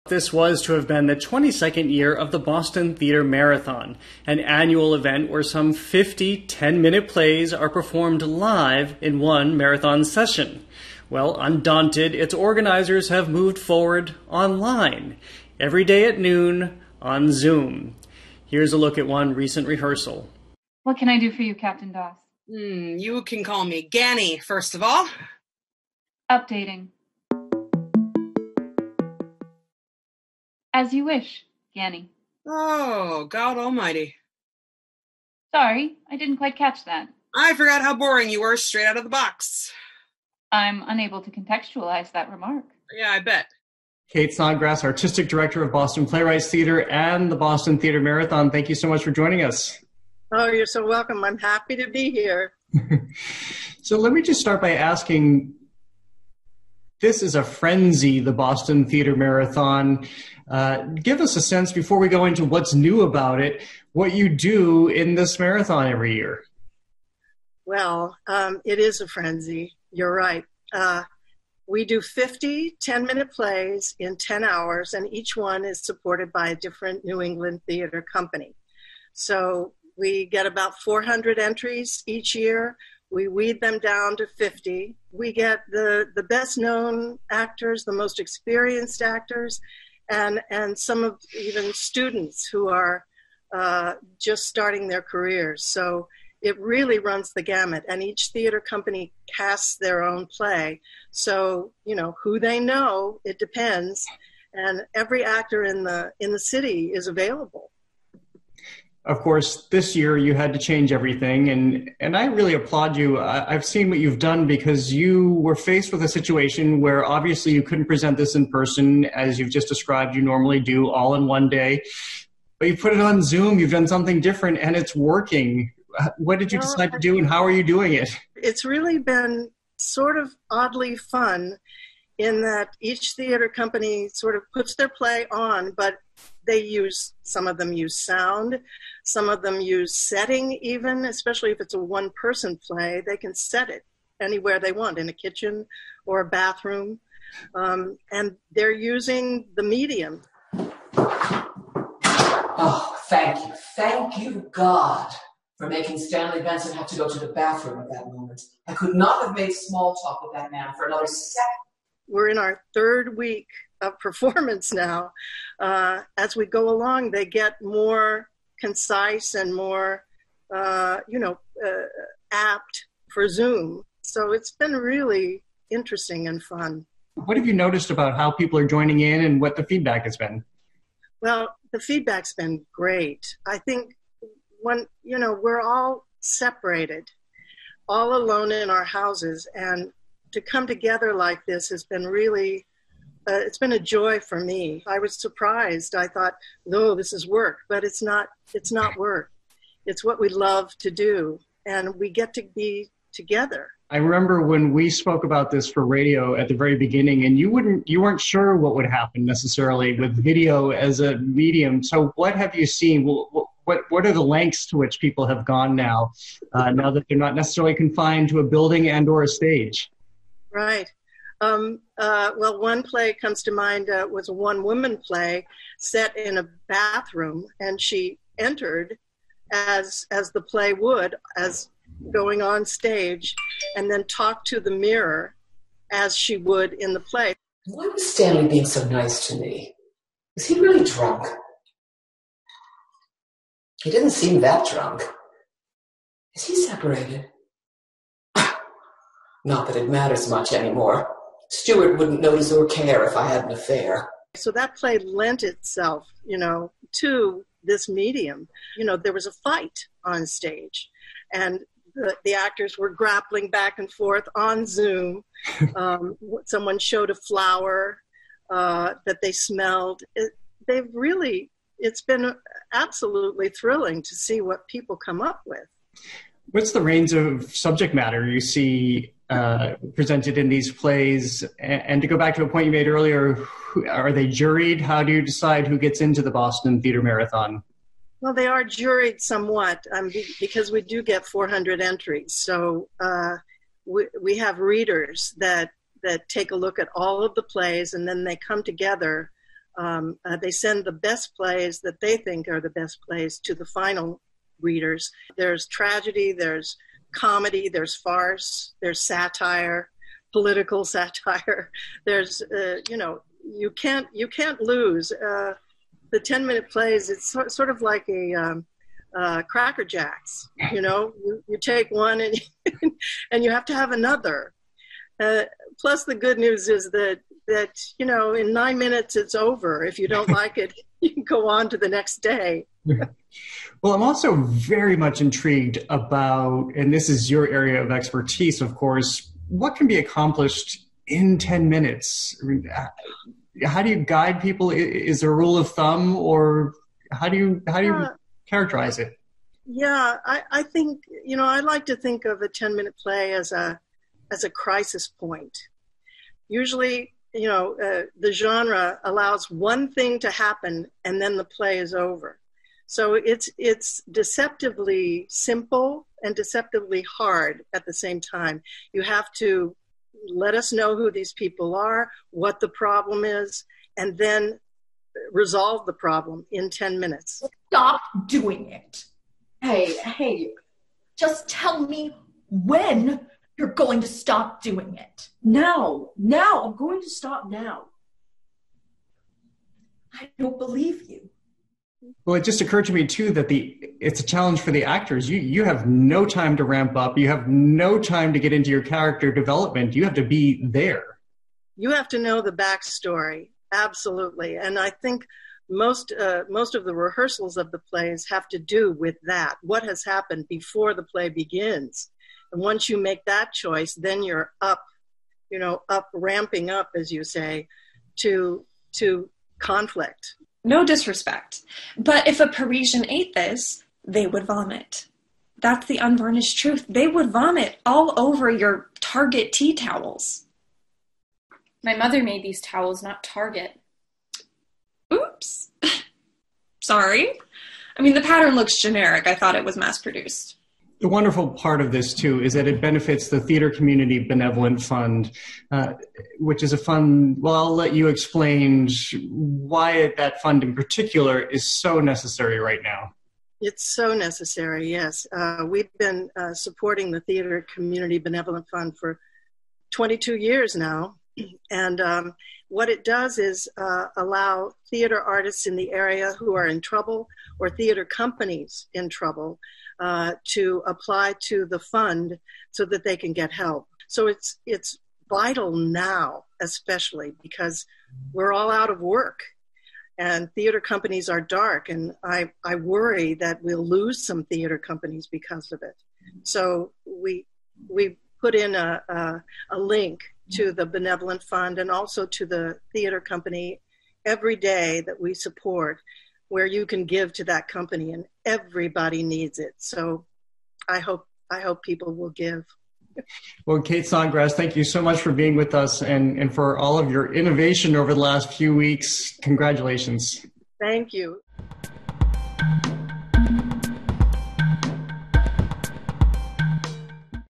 The Boston Theater Marathon Goes Virtual
The annual Boston Theater Marathon benefit presents their work—online.